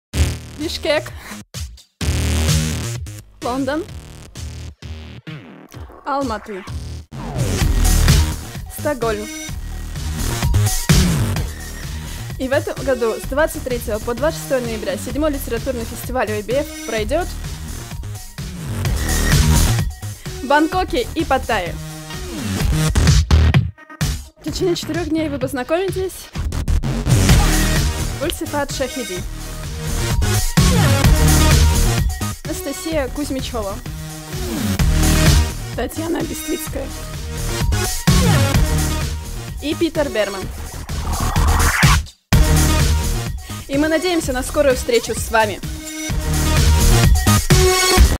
7th OEBF and Literary festival 2018 / 7-ой открытый евразийский фестиваль 2018
A new Russian voiced PROMO for the upcoming OEBF and Literary festival

7-ой открытый евразийский фестиваль 2018 промо на русском языке